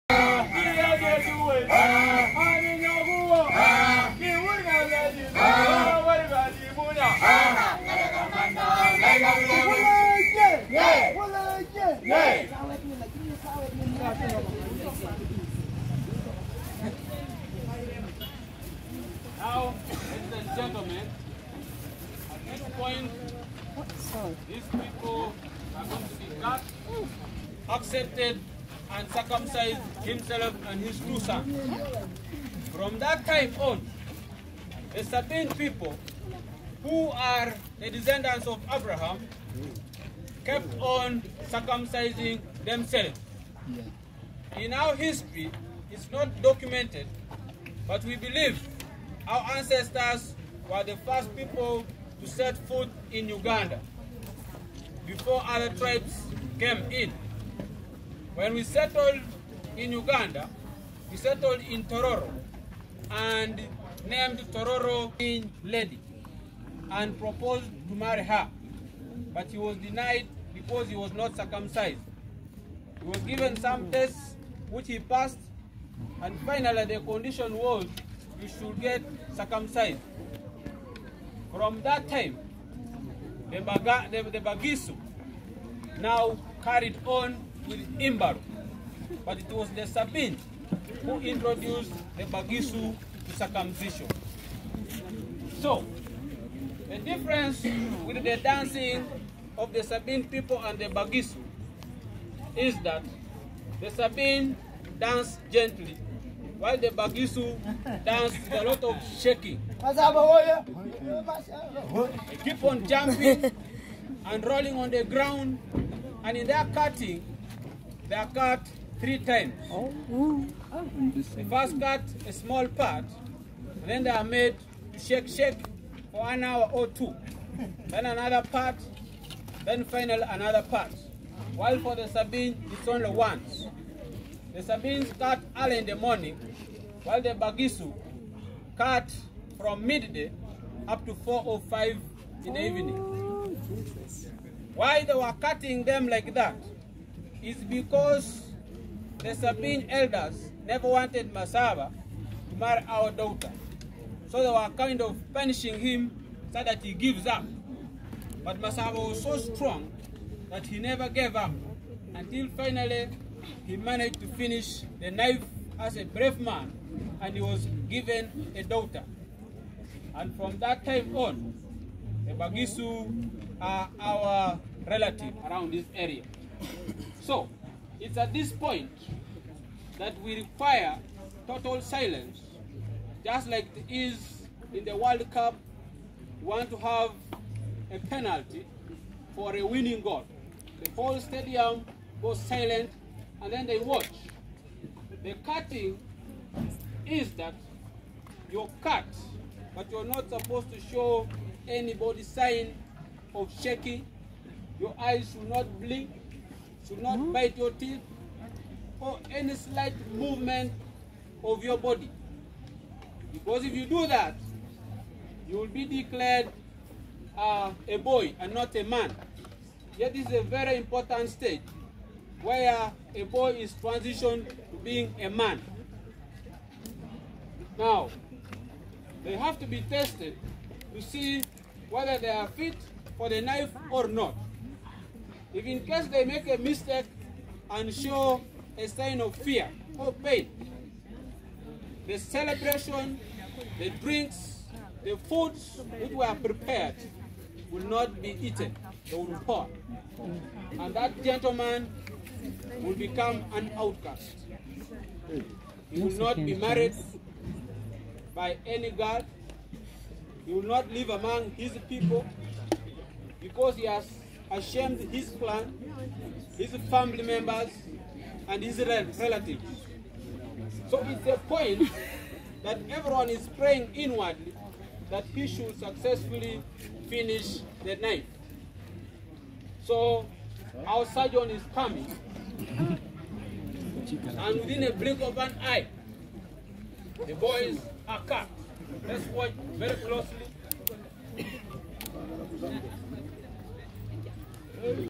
SABIN carry out circumcision in a special way
Zingayaffe experience is proudly showing how SABIN PEOPLE carry out their circumcision